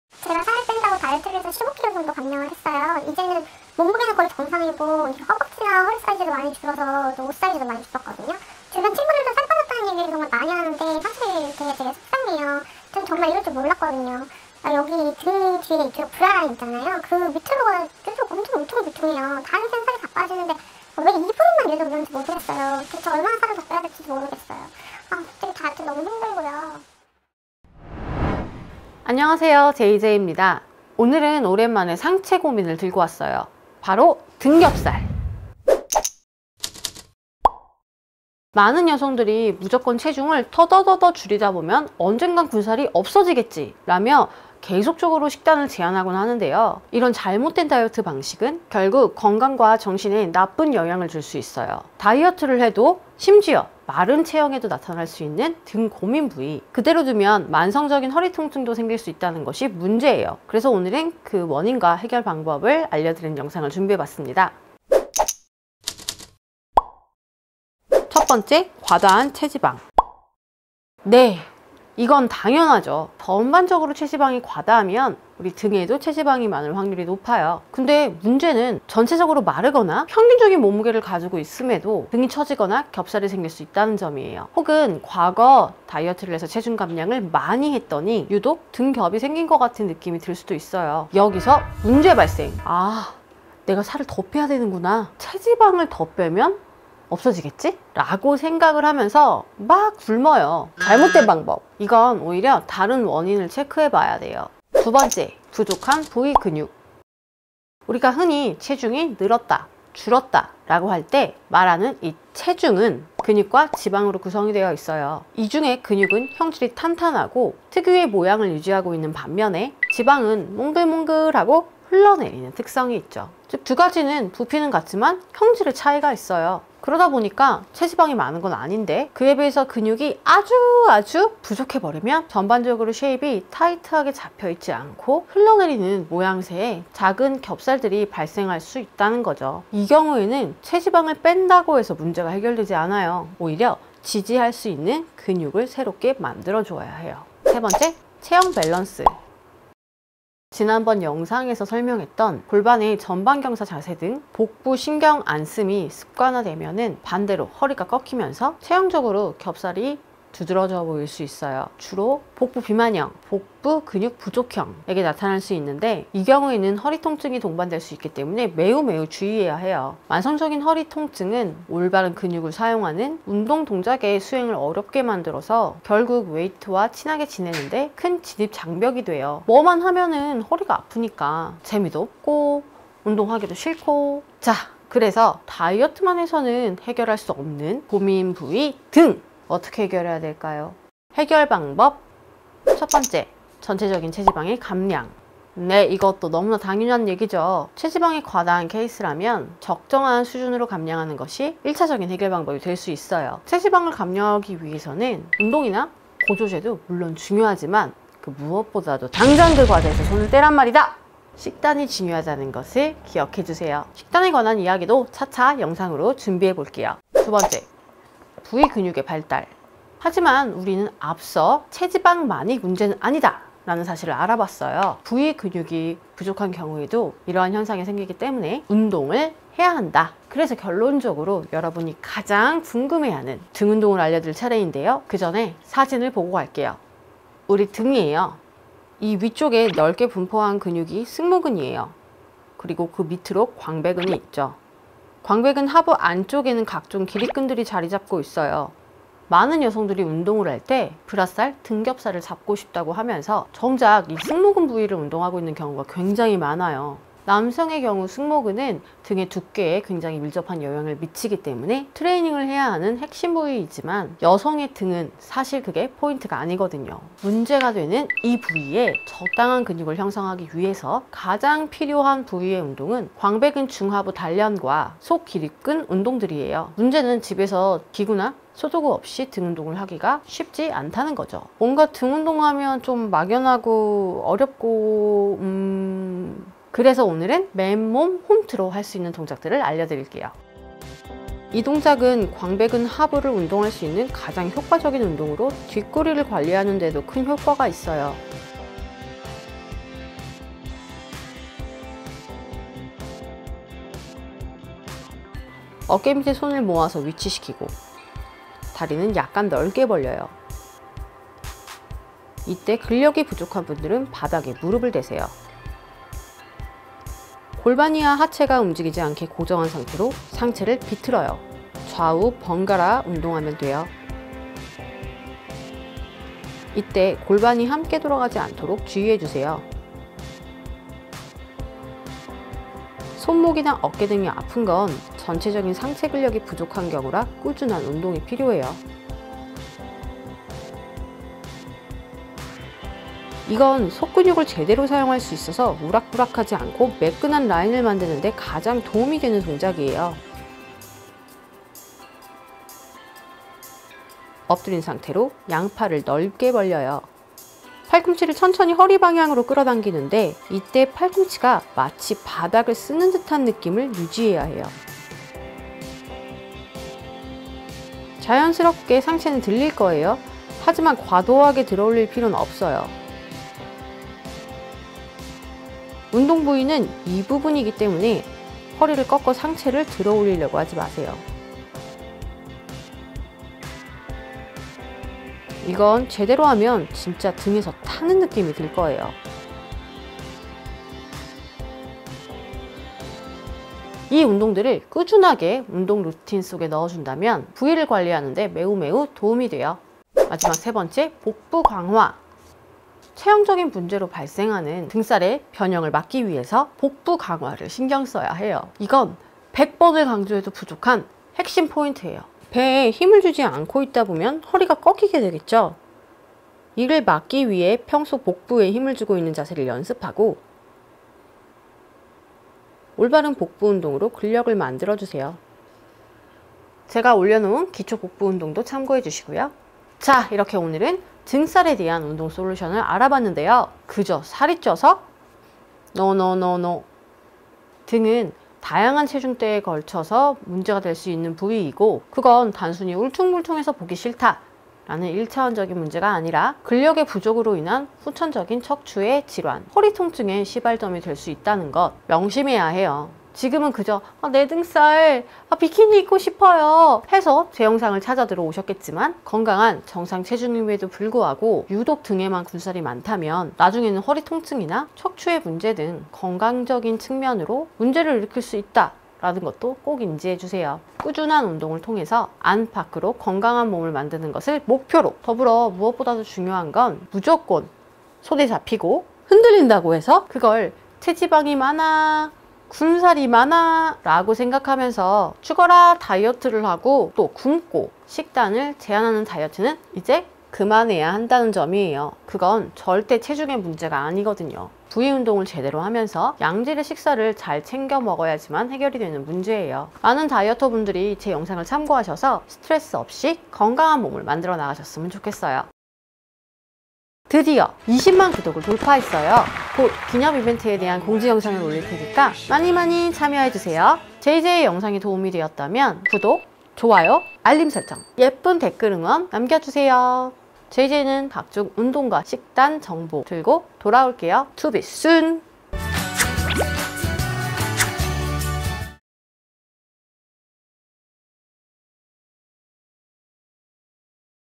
등겹, 브라살 원인과 해결. 도구x 홈트로 하는 등살빼는 운동. 다이어트를해도 울퉁불퉁한 등 왜그런건가요?
안녕하세요~ 제이제이입니다!
이번 영상은 등! 뒤태!가 고민인 분들을 위해 준비했어요
날씨가 더워지면서 얇은 상의를 입었을때 자꾸만 신경쓰이는 부위..
마냥 다이어트를 열심히 하거나 더 많이 굶는다고 해서 해결되지 않죠.
건강한 부위 관리법 알려드려요~

** 추천 + 구독 + 댓글은 사랑입니다!